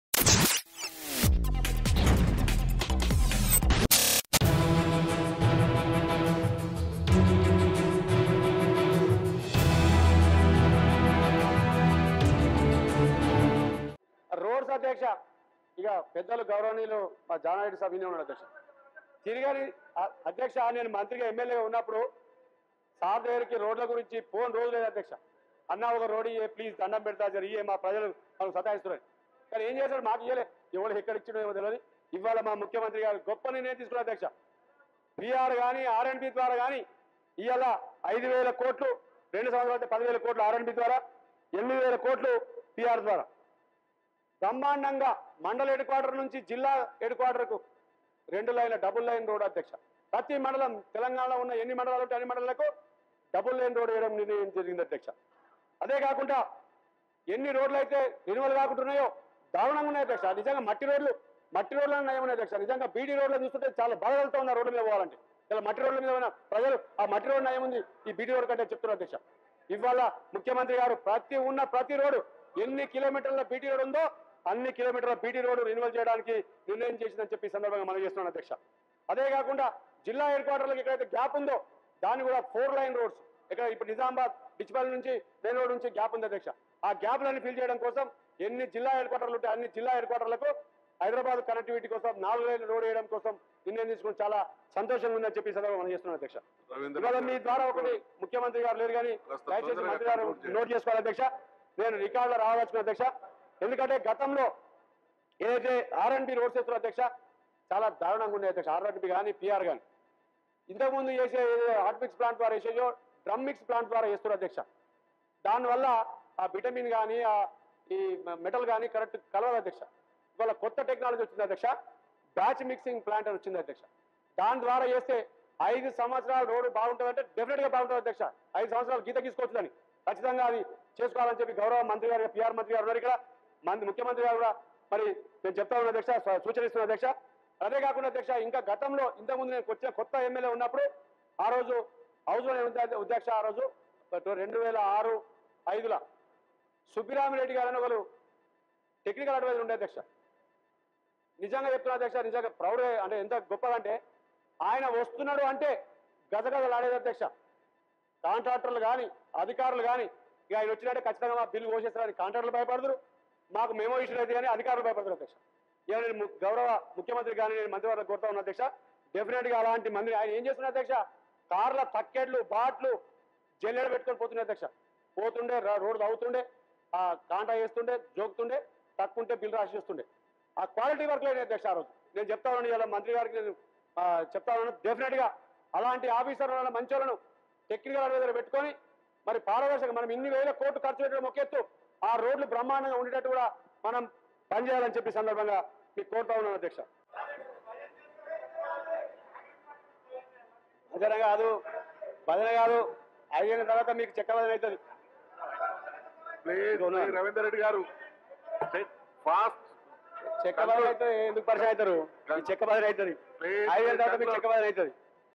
TRS MLA Ravinder Reddy praises Subbarami Reddy in Telangana Assembly
TRS MLA Ravinder Reddy has heaped praises on Subbarami Reddy over his work as Technical Advisor and requested government to appoint someone as him to inspect quality of roads in the state.